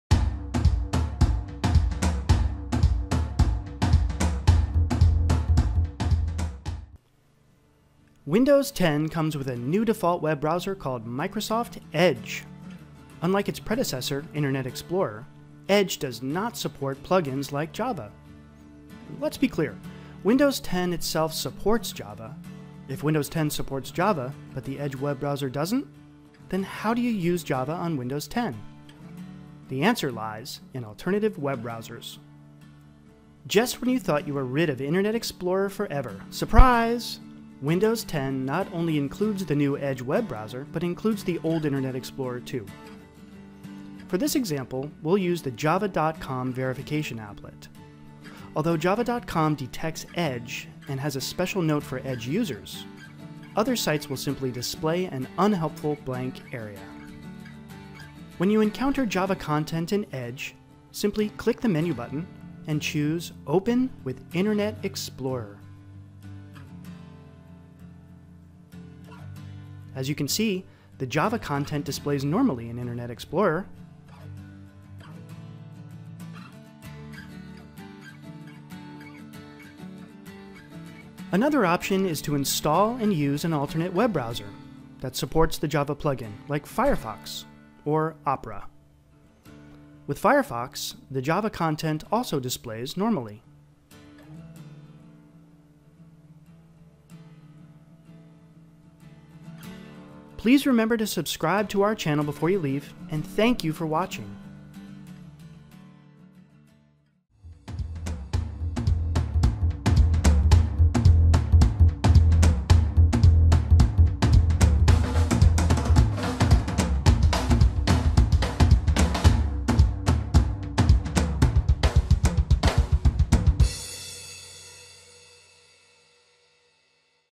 How to use Java on Windows 10
This tutorial demonstrates how to use Java on Windows 10.